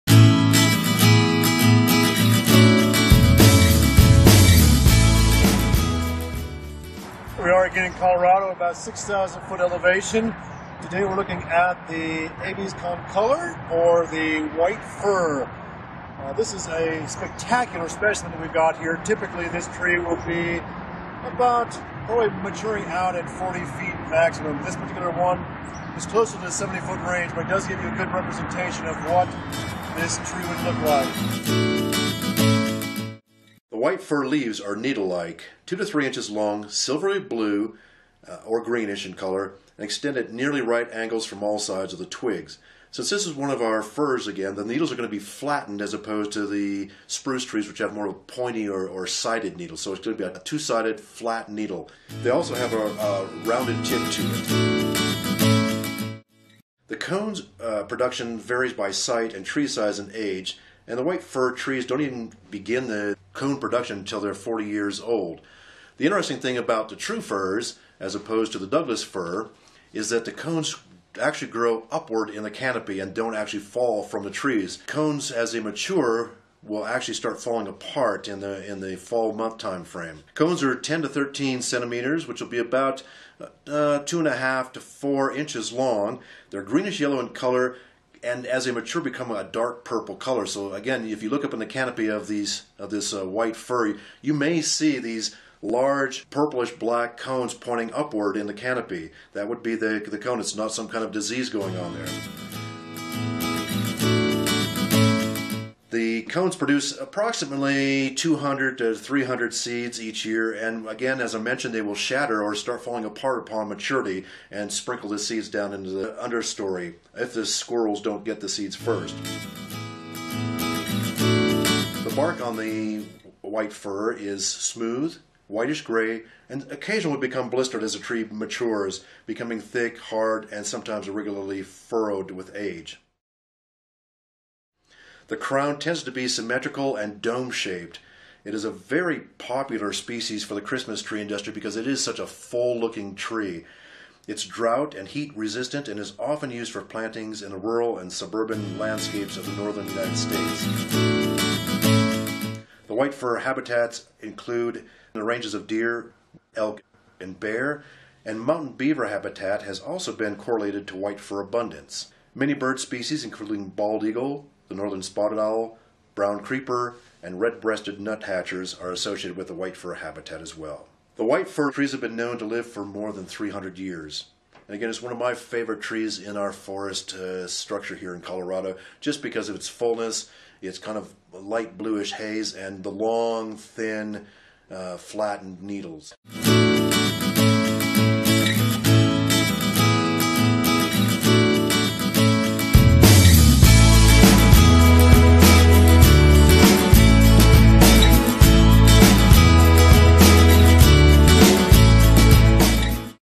PlantTalk: White Fir
Mark Platten, Director of Teller County Extension, discusses the White Fir.